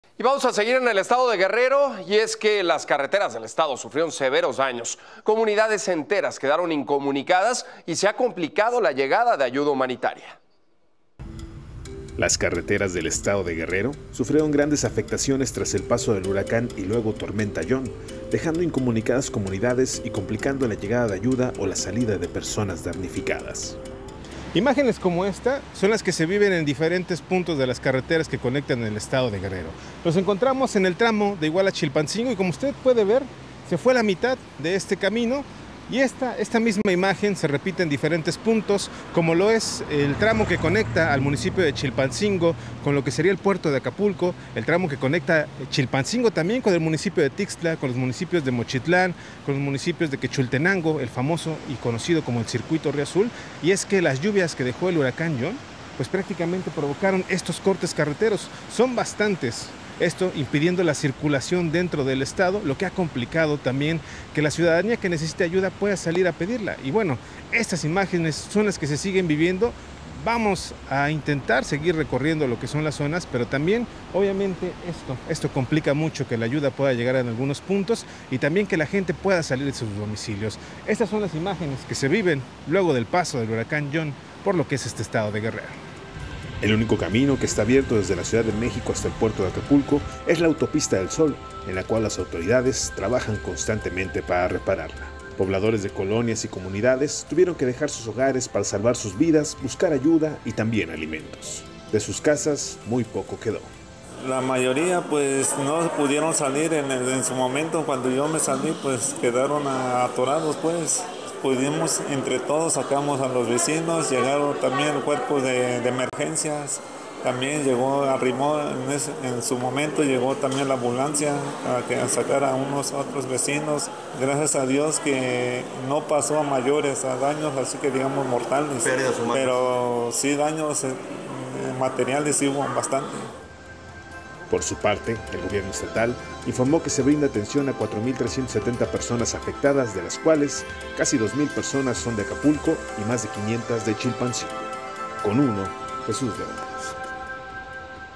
Comunidades de Guerrero quedan incomunicadas por daños en carreteras.
Debido a las intensas lluvias de la última semana, la Autopista del Sol es la única vía abierta para conectar Acapulco con la Ciudad de México, el resto de las carreteras en torno al puerto y a Chilpancingo, la capital del estado, quedaron intransitables.